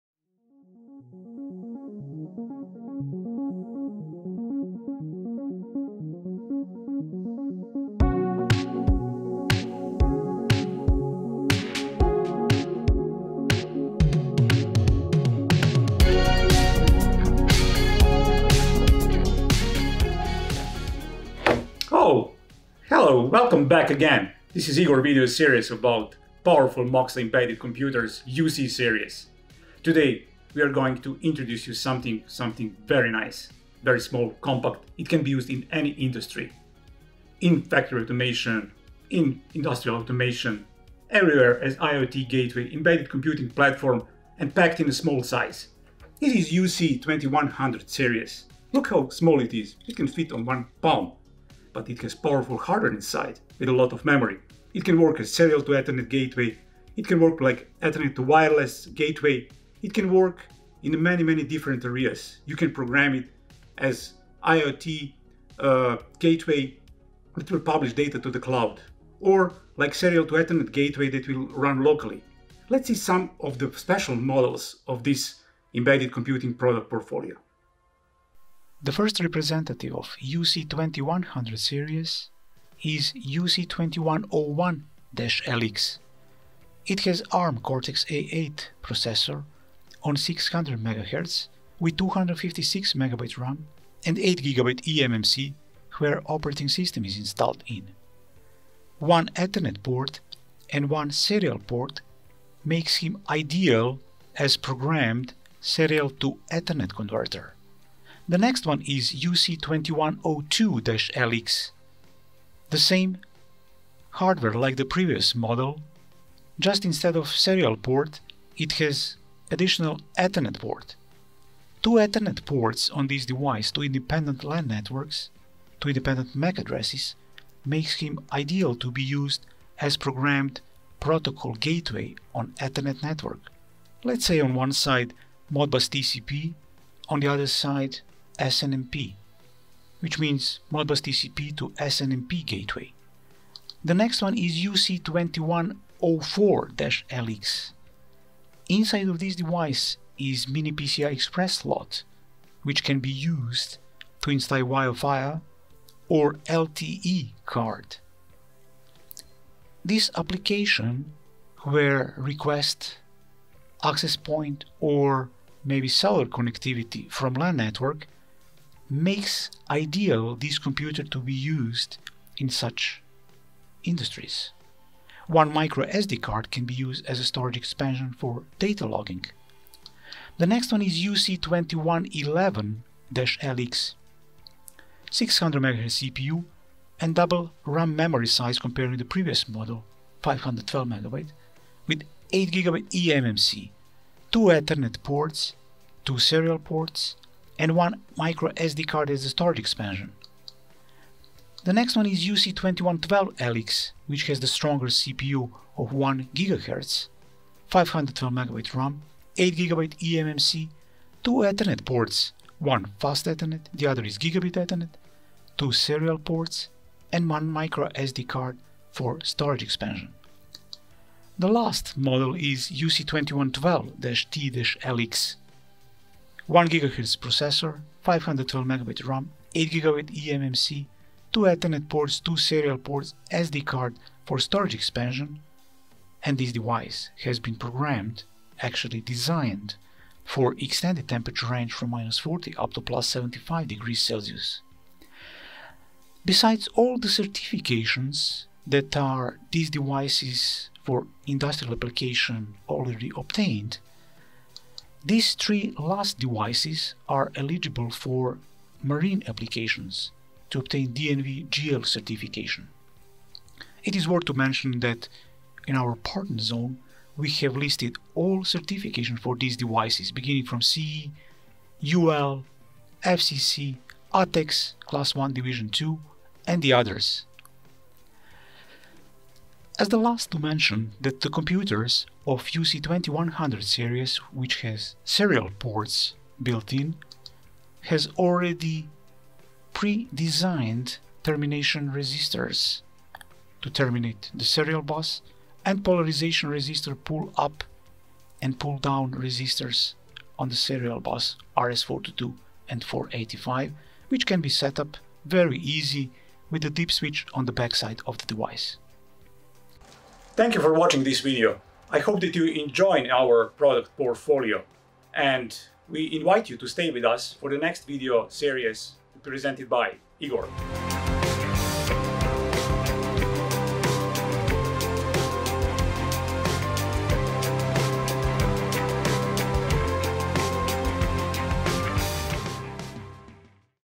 Powerful Palm-sized IIoT Gateway
In this video, Igor will give a comprehensive introduction to the UC-2100 Series product portfolio.

This computing platform is perfect for embedded data acquisition and processing applications. The UC-2100 Series has a built-in Cortex-A8 Arm-based processor that has been optimized for a variety of industrial and IoT solutions. With its flexible interface options, this tiny embedded computer is a reliable and secure gateway for data acquisition and processing at field sites and is a useful communication platform for many other large-scale deployments. Models designed for wide temperature applications are available for use in extreme environments such as those found in the oil and gas industry.